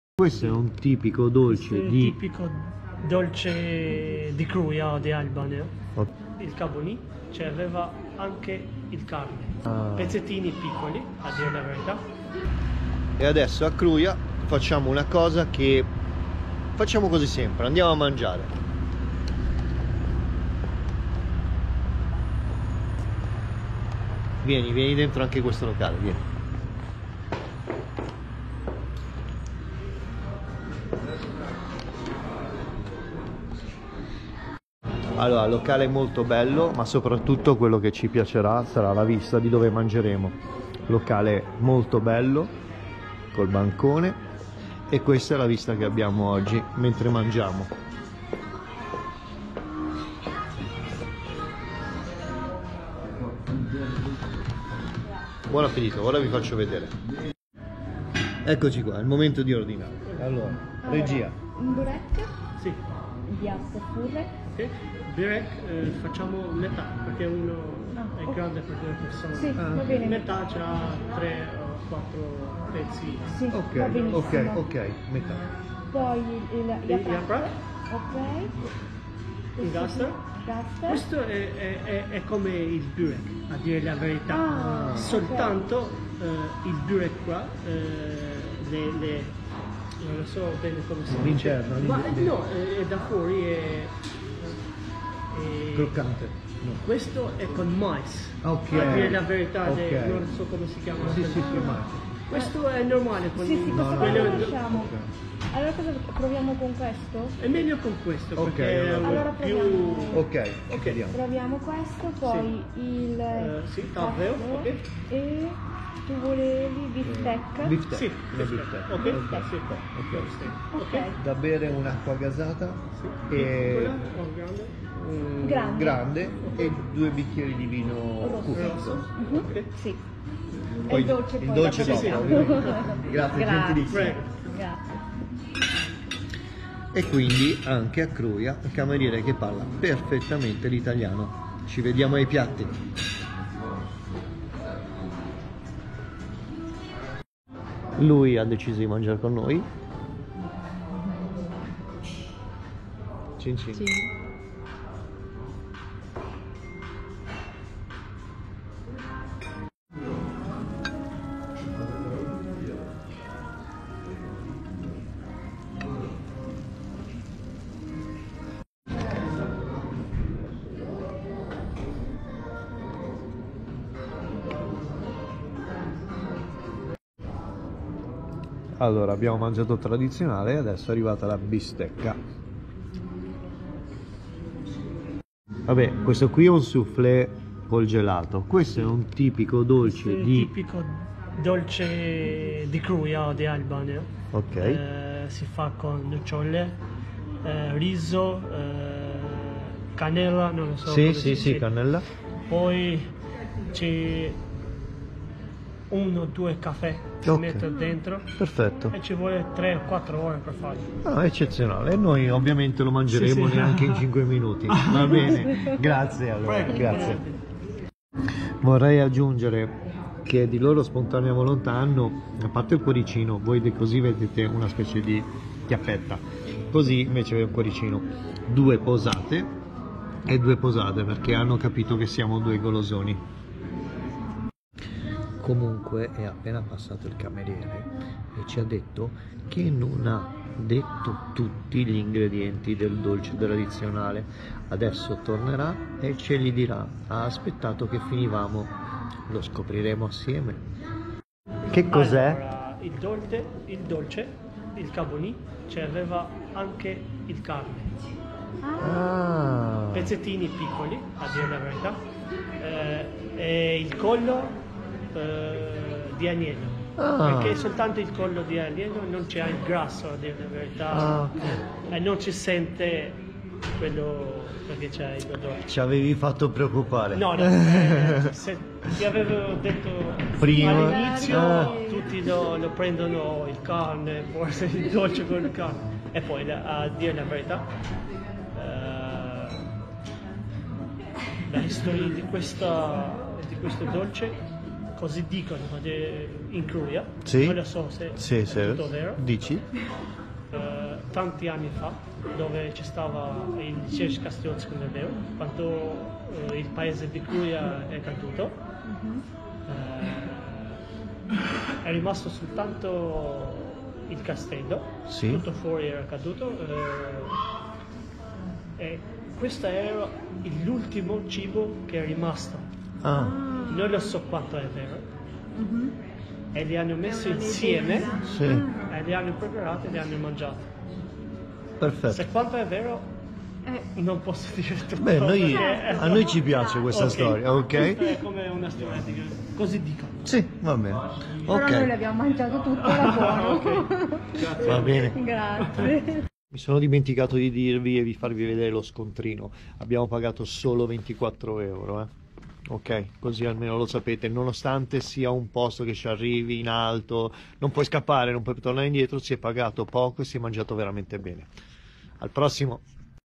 Pranzo tradizionale a Kruja🇦🇱
Ciao sono Max e vivo in Albania da oltre 3 anni, oggi ti faccio vedere un altro tipico pranzo che potrai fare qui in Albania nello specifico a Kruja